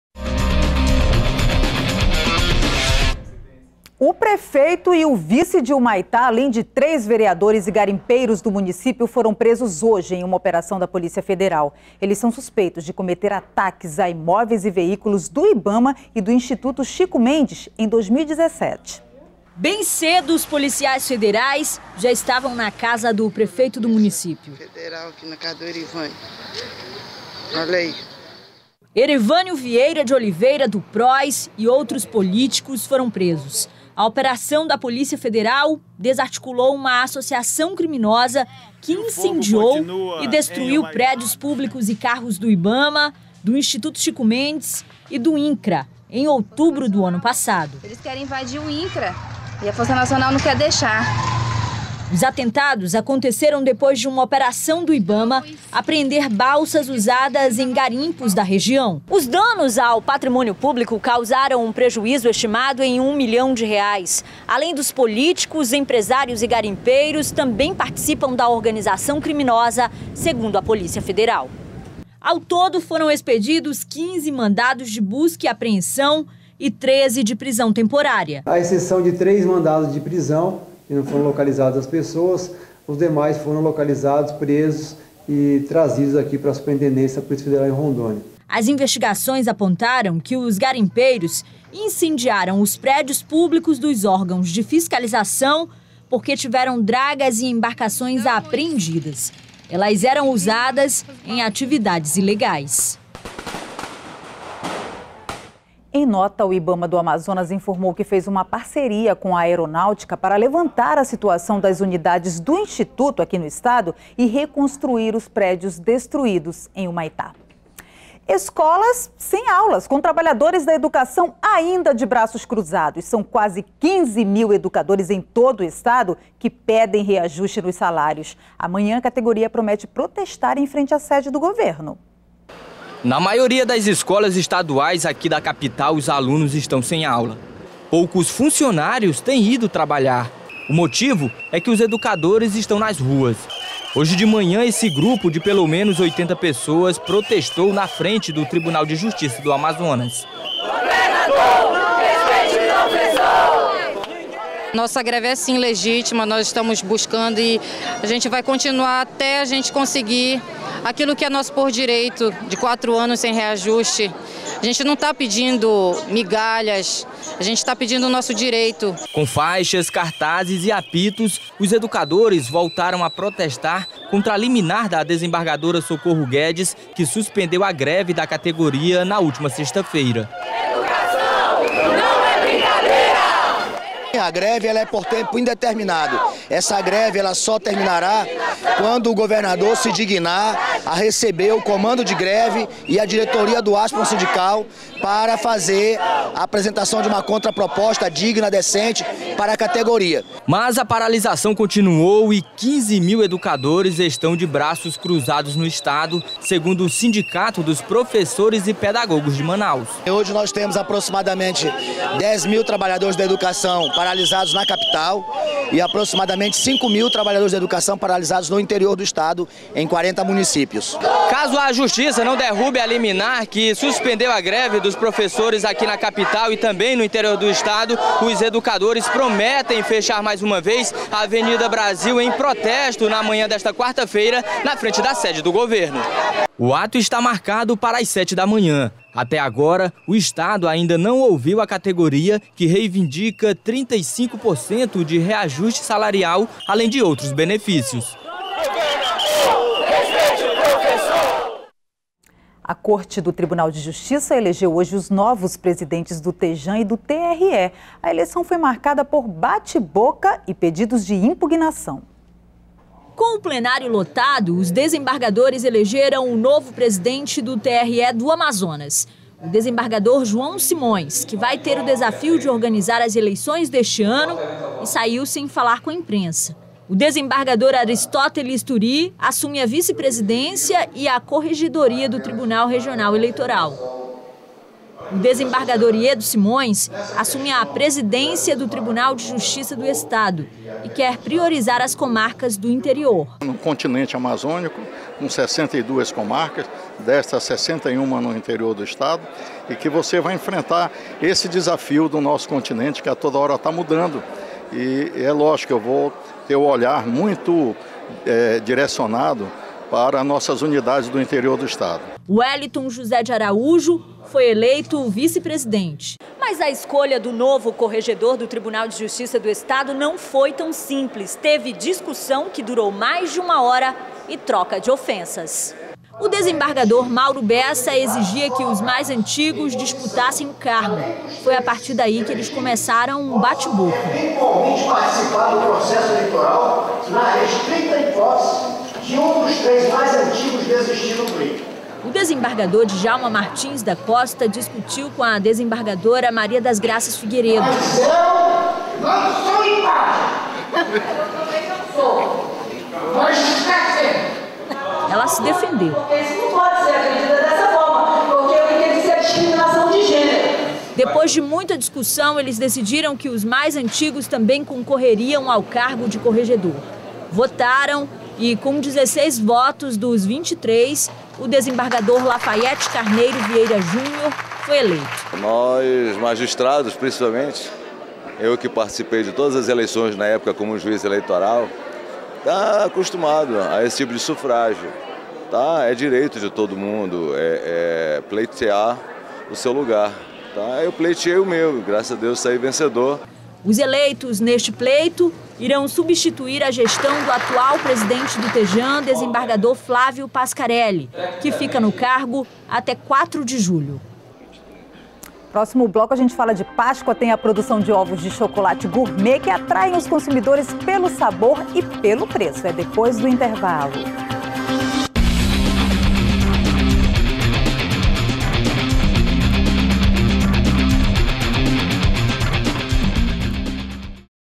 Operação da Polícia Federal prende o prefeito de Humaitá, o vice e três vereadores do município
02º BLOCO JORNAL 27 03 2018 - Operação da Polícia Federal prende o prefeito de Humaitá, o vice e três vereadores do município